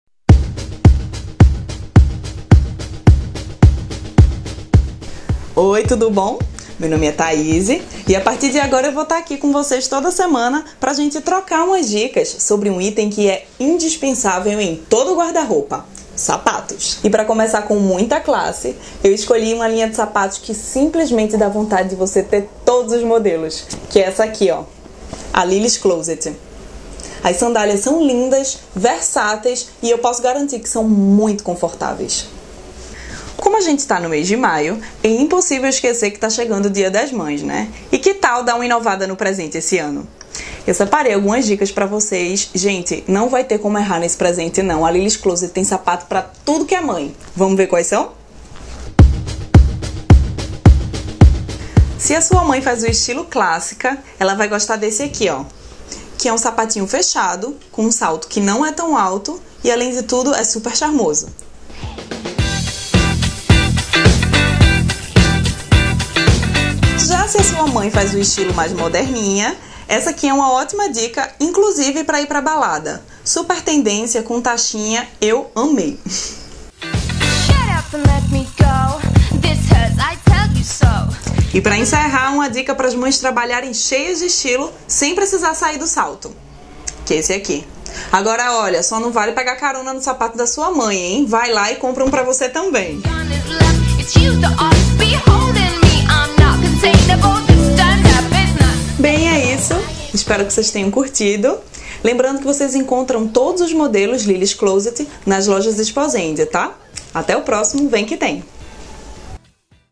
Vem Que Tem - Lilly's Closet
Os sapatos da Lilly's Closet  não passam despercebidos: são do tipo que fazem você querer entrar na loja, experimentar e depois não tirar mais do pé. Nós do VQT aproveitamos para dar dicas de presente para o Dia das Mães!

Acessa nosso blog? :D